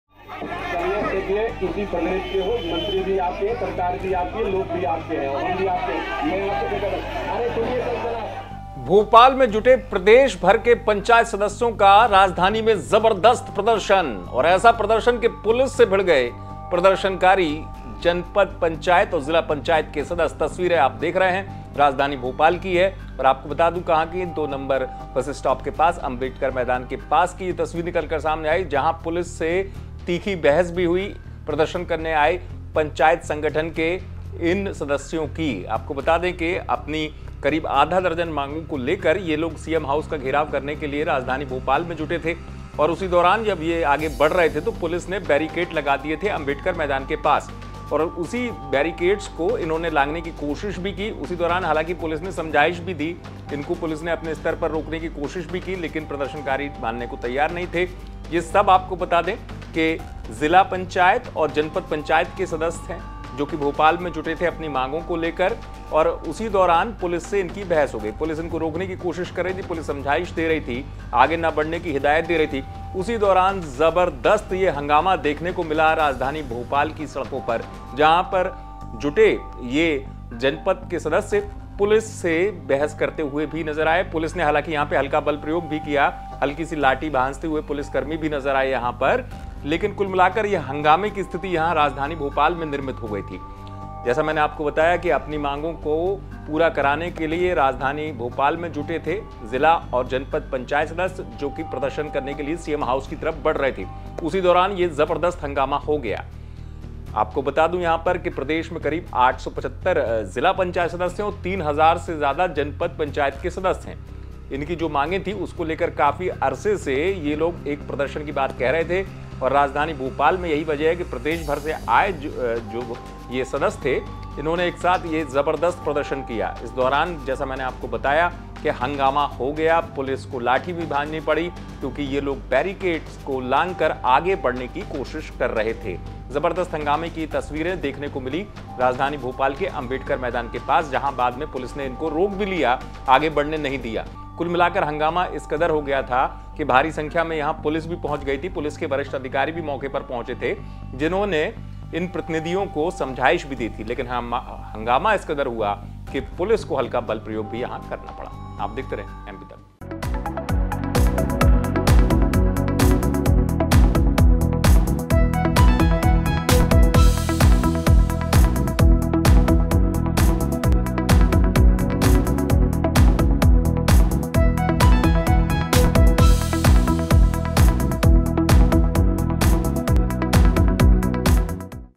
Bhopal में सीएम हाउस का घेराव करना पड़ गया प्रदर्शनकारियों को भारी, पुलिस ने लाठी से खदेड़ दिया !
Bhopal में सीएम हाउस का घेराव करना पड़ गया प्रदर्शनकारियों को भारी, पुलिस ने लाठी से खदेड़ दिया ...दोखिए पूरी रिपोर्ट...